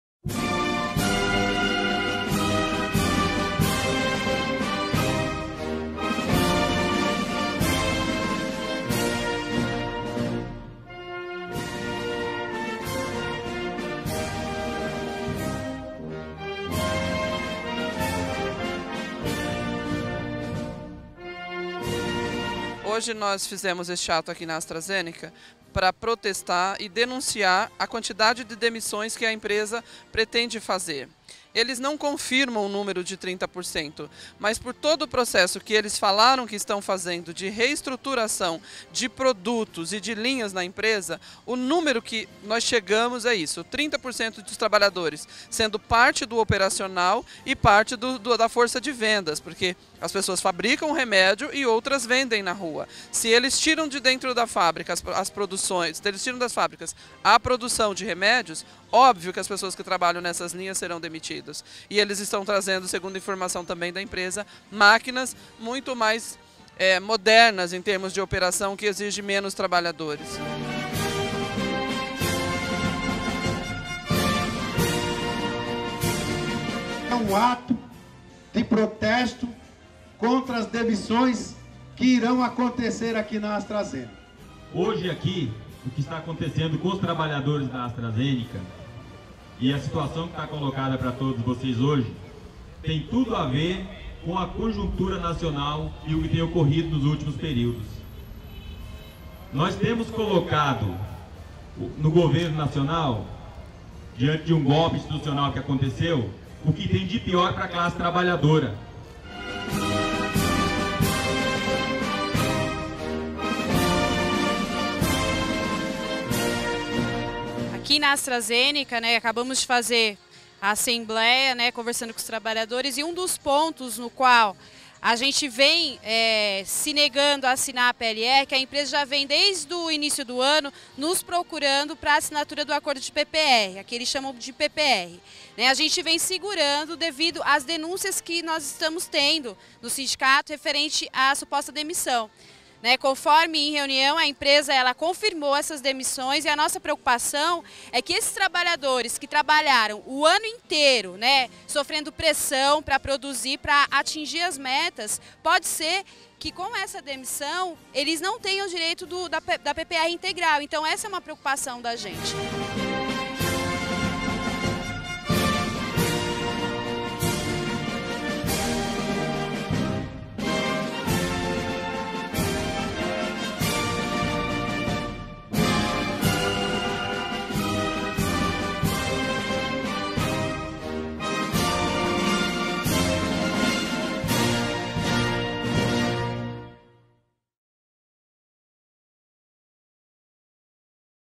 Astrazeneca: demissões em massa na unidade de Cotia
Na segunda semana de novembro, chegaram ao sindicato denúncias de que a Astrazeneca demitiria 30% dos funcionários. O sindicato imediatamente tomou a iniciativa de solicitar reunião com a empresa para apurar o caso. Apesar da solicitação ter sido feita no dia 11 de novembro, a empresa agendou reunião somente para o dia 17, demonstrando mais uma vez sua desumanidade e descaso.